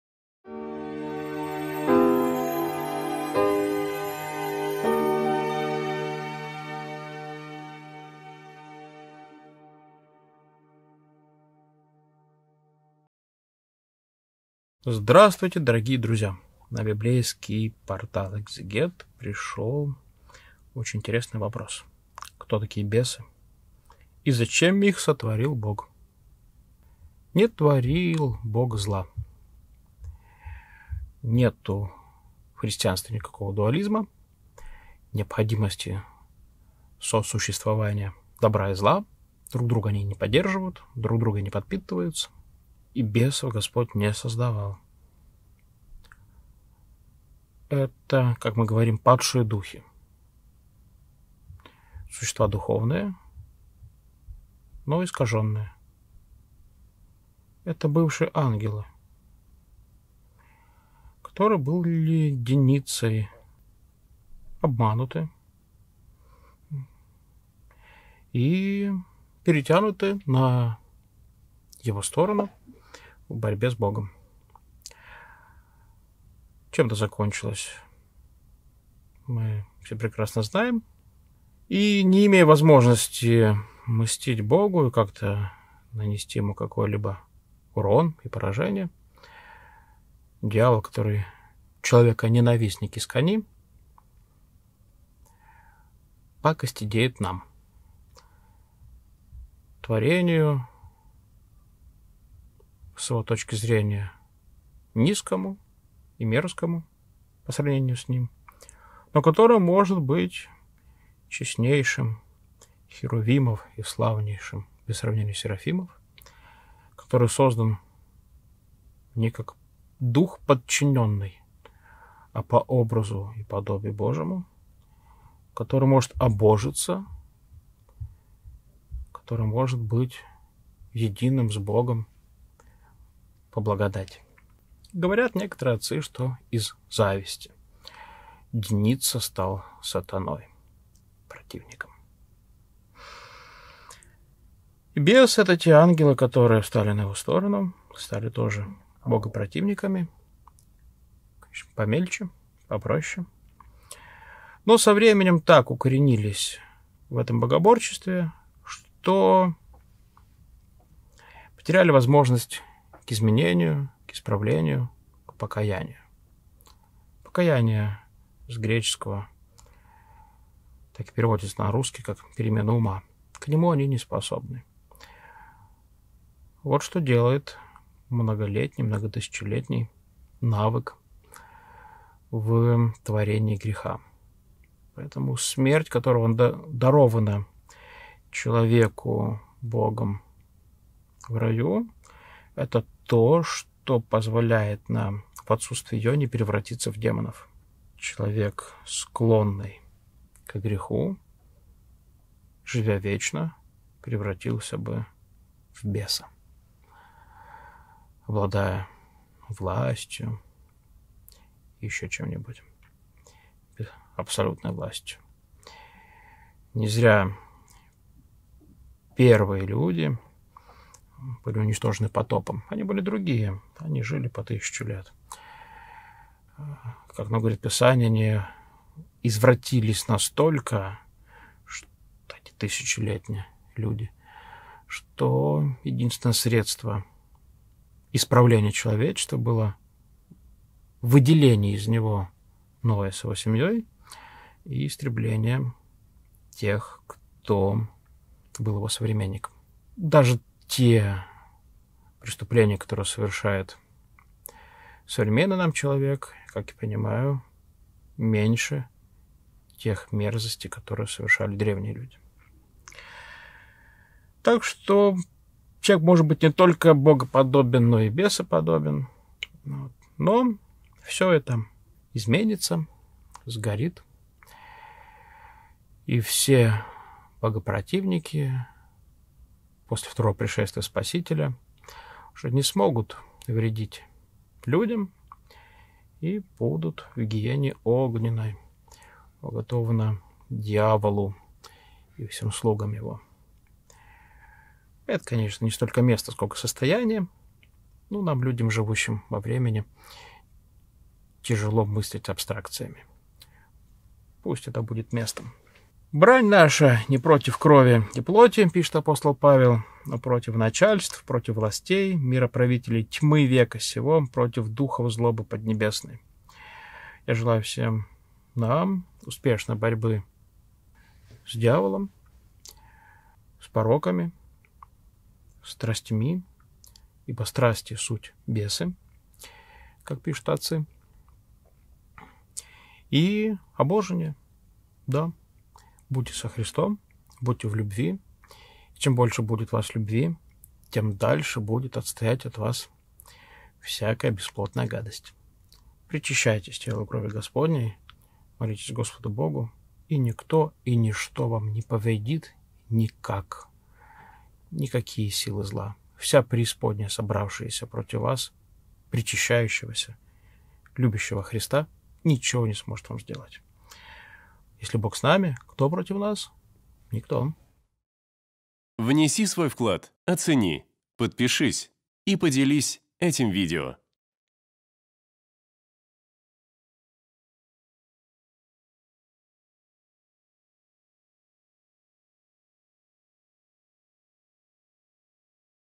КТО ТАКИЕ БЕСЫ И ЗАЧЕМ ИХ СОЗДАЛ БОГ ? Священник Владислав Береговой
Команда Экзегет.

Библейский портале Экзегет вместе со священником Владиславом Береговым отвечает на вопрос: "Кто такие бесы и зачем их создал Бог?"

🔸   Бог не создавал зла
🔸   Бесы – падшие духи
🔸   Грехопадение ангелов
🔸   Богоборчество отпавших от Него
🔸   Борьба с пороками и страстями
🔸   "Если Бог за нас, кто против нас?"

📜 Читать Послание к Ефесянам главу 6, стих 12 (с толкованиями)